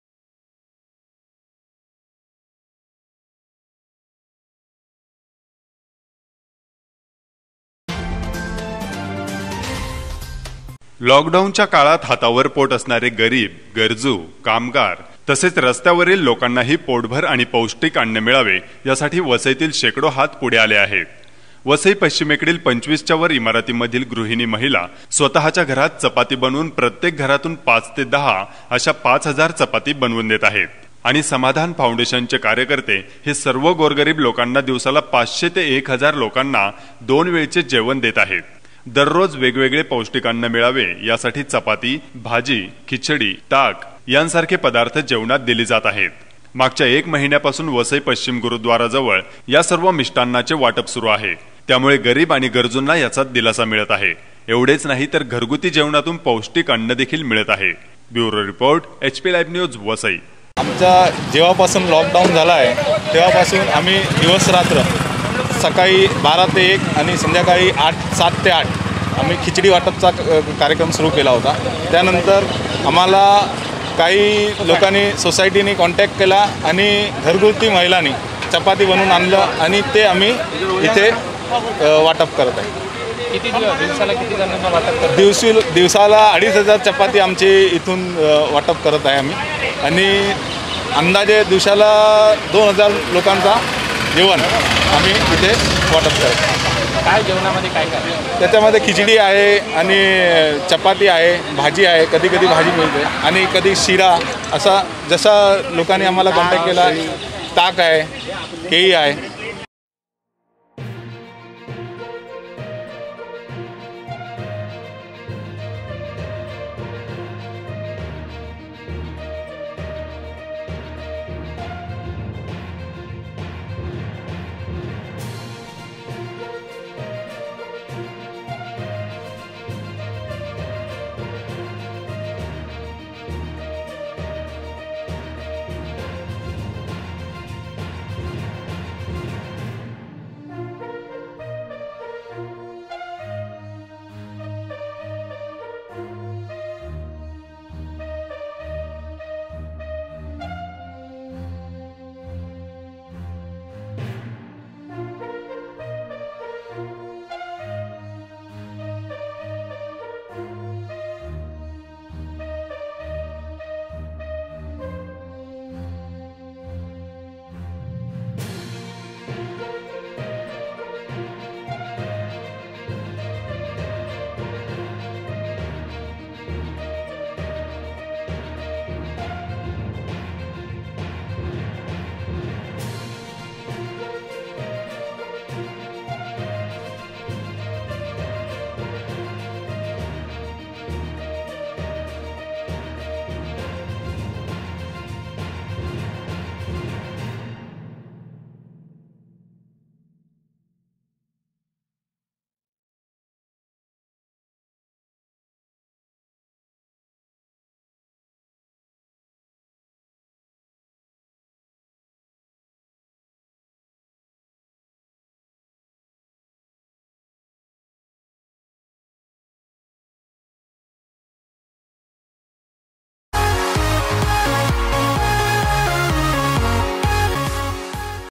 Sunshine story: Vasai women roll out 5,000+ bread to feed hunger
लॉक डाऊन च्या काळात हातावर पोट असणारे गरीब, गरजू, कामगार, रस्त्यावरील लोकांनाही पोटभर आणि पौष्टिक अन्न मिळावे यासाठी वसईतील शेकडो हात पुढे आले आहेत. वसई पश्चिमेकडील 25 च्या वर इमारतीमधील गृहिणी महिला स्वताच्या घरात चपाती बनवून प्रत्येक घरातून 5 ते 10 अशा 5000  चपाती बनवून देत आहेत.. आणि समाधान फाउंडेशन हे सर्व गोरगरीब लोकांना दिवसाला 500 ते 1000 लोकांना 2 वेळचे जेवण देत आहे. दररोज वेगवेगळे पौष्टिक अन्न मिळावे यासाठी चपाती, भाजी, खिचडी, ताक, केली या जेवणात दिली जात आहे. मागच्या एक महिन्यापासून वसई पश्चिम गुरुद्वारा जवळ याचे वाटप सुरू आहे. त्यामुळे गरीब गराजुना याचा दिलासा मिळत आहे. एवढेच नाही तर घरगुती जेवणातून पौष्टिक अन्न मिळत आहे